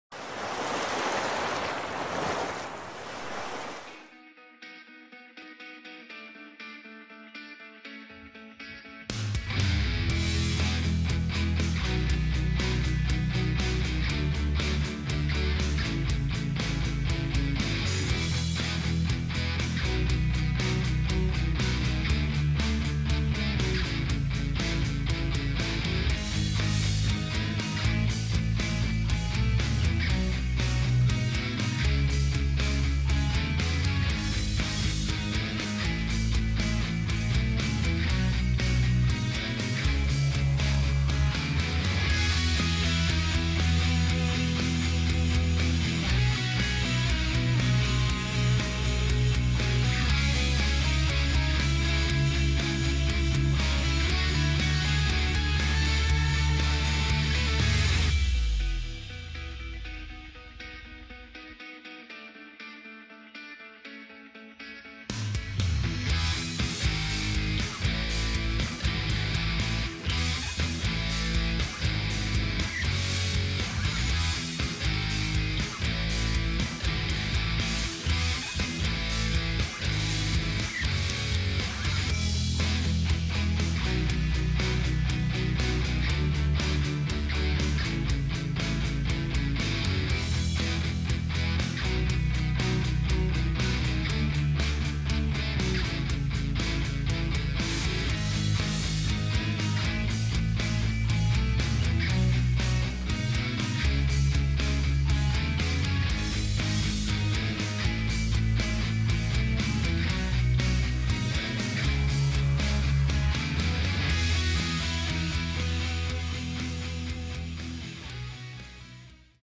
Escuela de Tabla Olas Perú inicia el verano 2021
Todo listo para el regreso de la escuela de surf pionera del Perú.